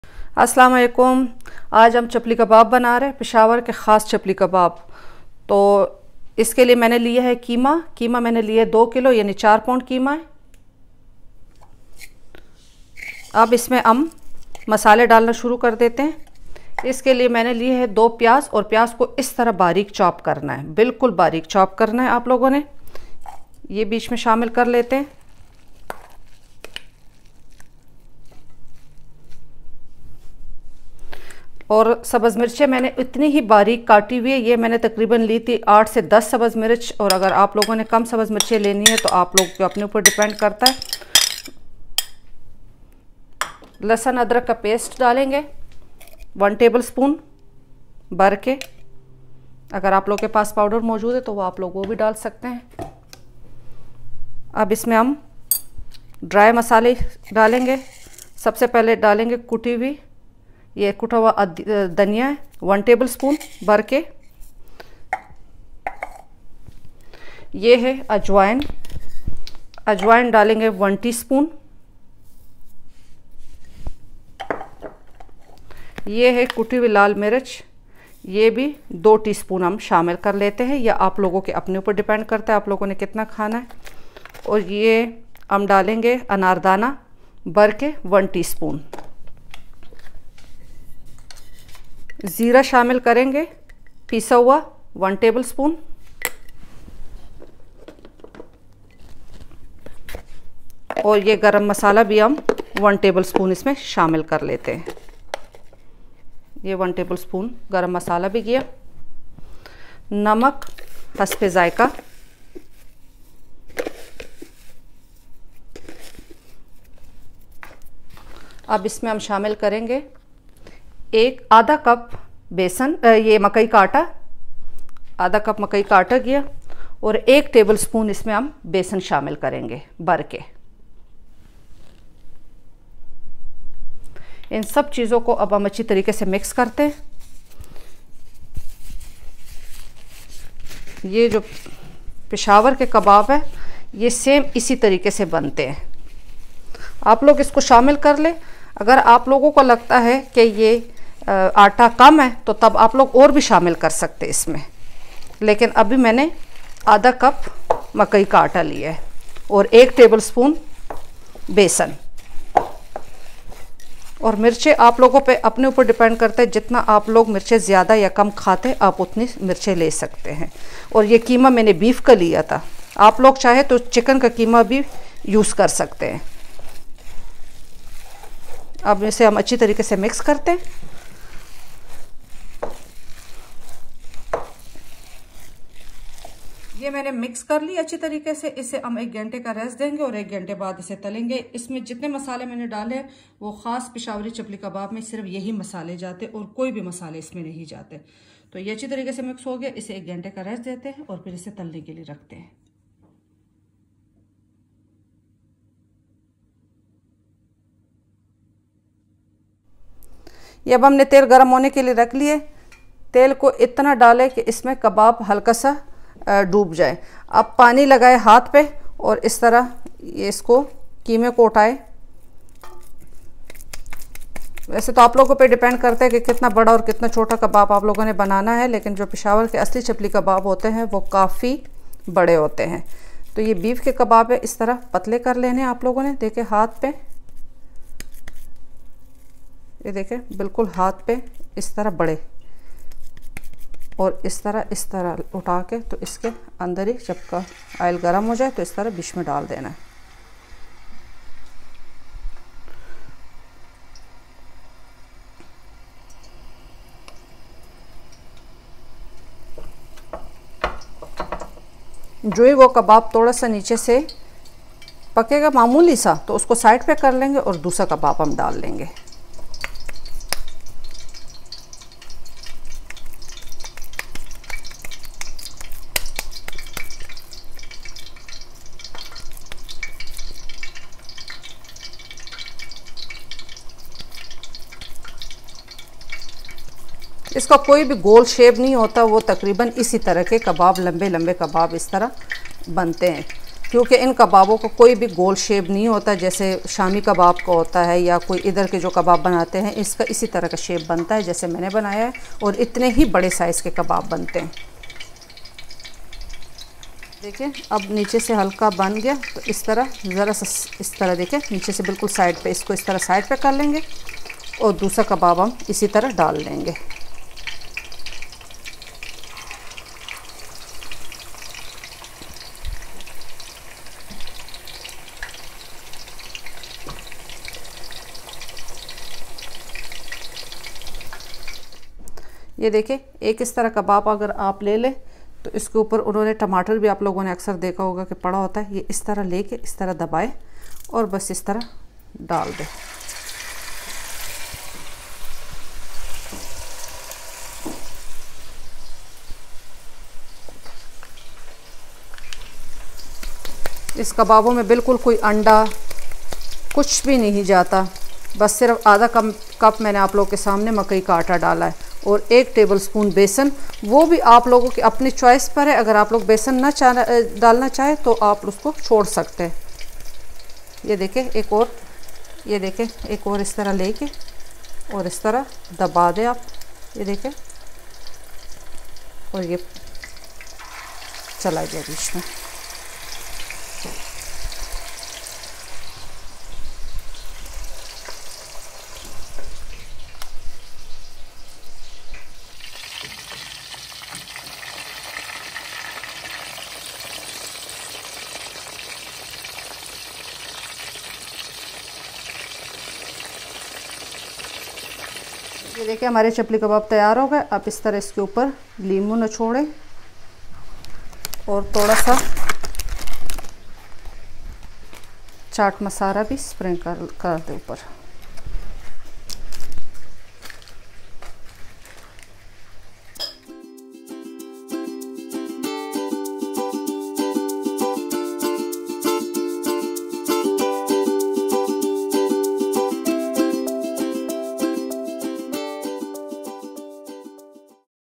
Special Street Style Peshawari Chapli Kebab | Restaurent Style Recipe
Hope you enjoy this authentic peshawari chapli kebab recipe.

Ingredients :

 4 Lb Beef Qeema
2 Medium Size Chooped Onion
8-10 Green Chilli
1 Tbsp Ginger Garlic Paste
1 Tbsp Crushed Corainder Powder
1 Tspn Ajwain
2 Tspn Crushed Red Chilli Powder
1 Tspn Anar Daana
1 Tbsp Garam Masala
1 Tbsp Zeera Powder
Salt to Taste
1/2 Maize Flour
1 Tbsp Besan